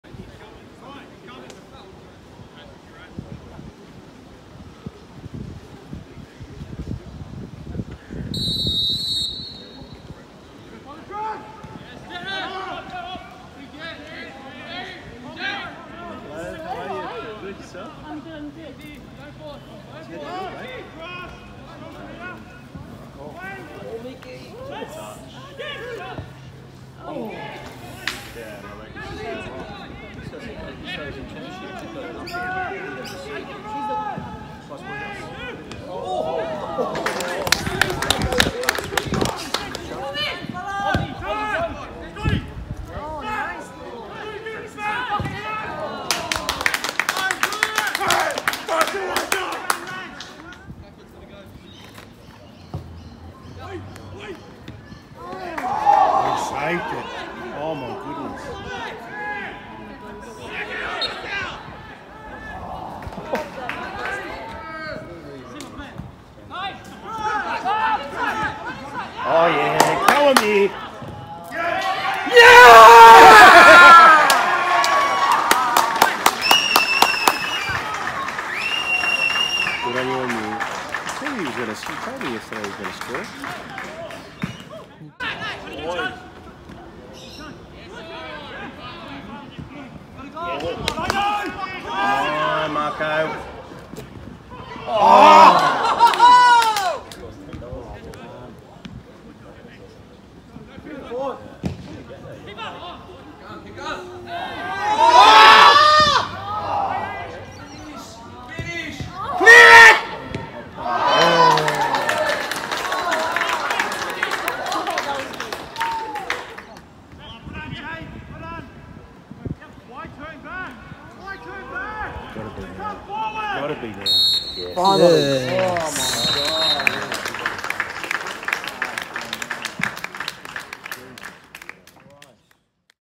HIGHLIGHTS | Sydney University SFC 0-1 Macarthur Rams | FNSW League Two - 2022 Round 2
Macarthur Rams travelled to Dulwich Hill to play Sydney University SFC in Round 2 of the Football NSW Men's League Two season!

🕖 - 3:00pm
🏟️ - Arlington Oval
📆 - 20/03/2022
🎥 - Macarthur Rams